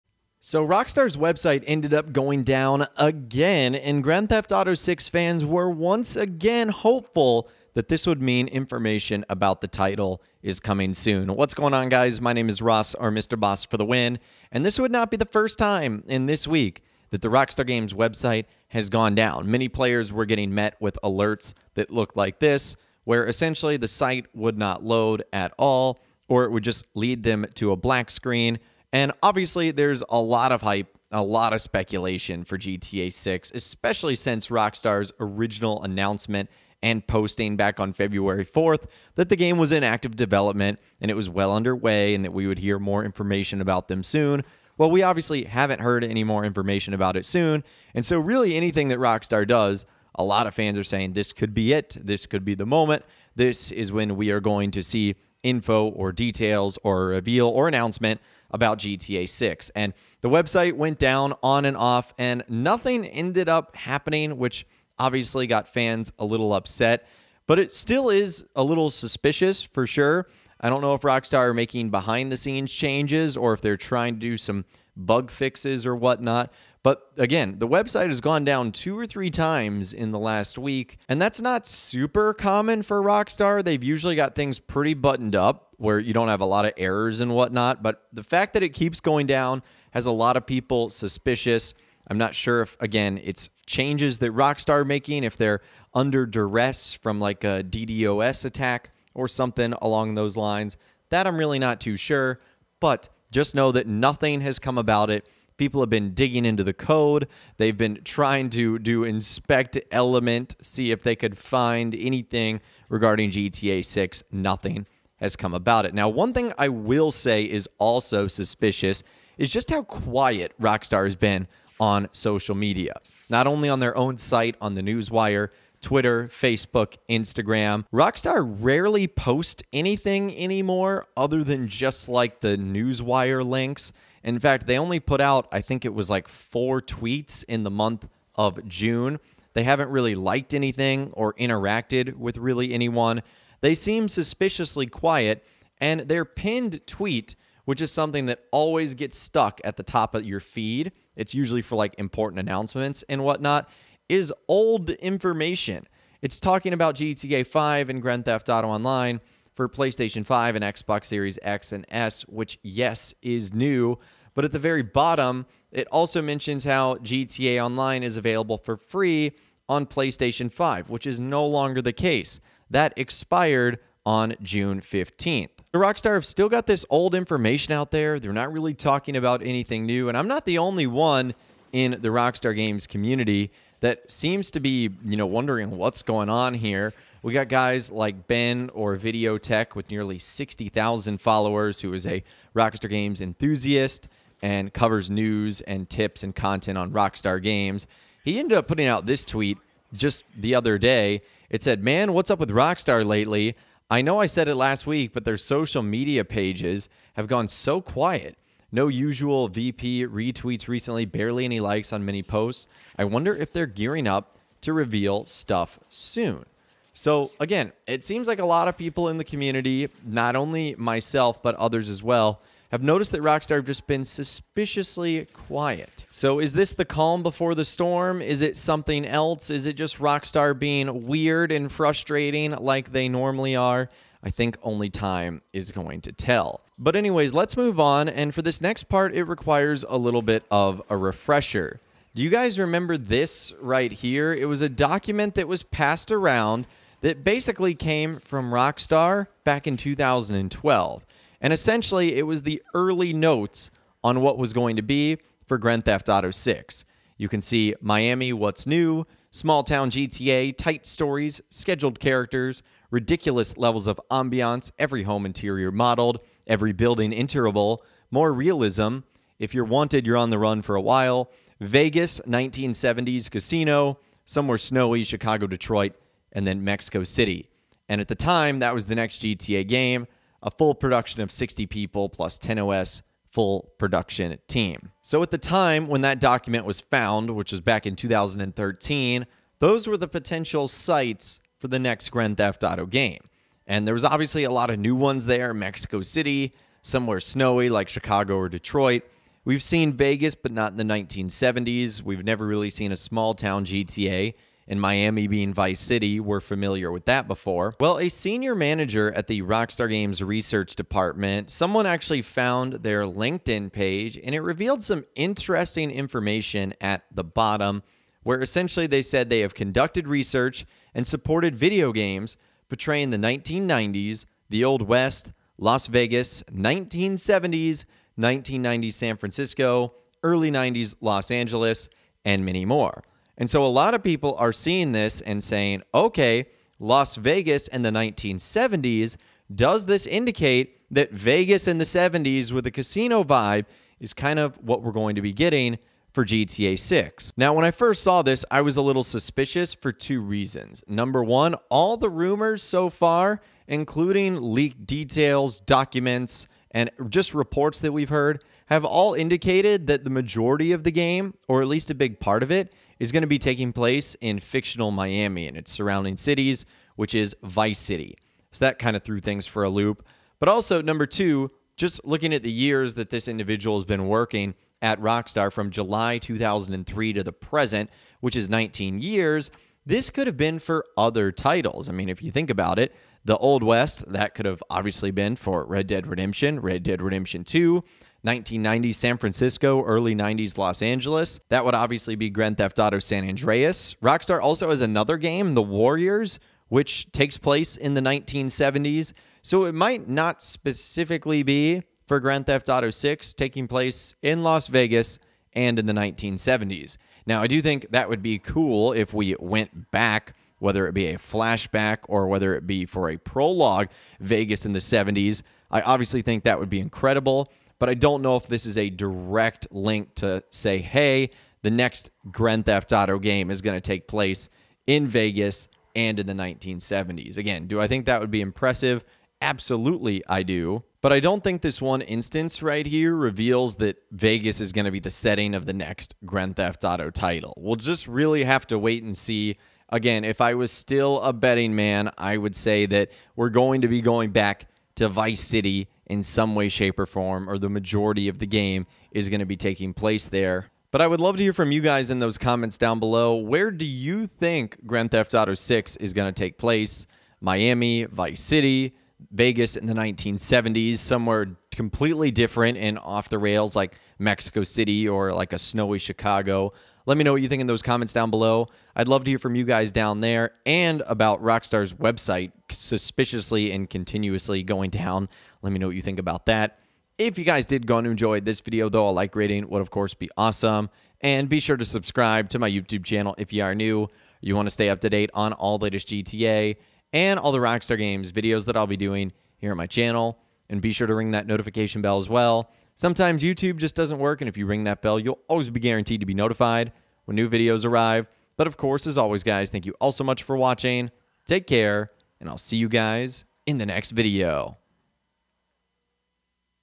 GTA 6...LAS VENTURAS In The 1970s? Rockstar Gaming Acting VERY Suspicious & MORE! (GTA VI)
In today’s Grand Theft Auto 6 video – GTA 6...LAS VENTURAS In The 1970s? Rockstar Gaming Acting VERY Suspicious & MORE! (GTA VI)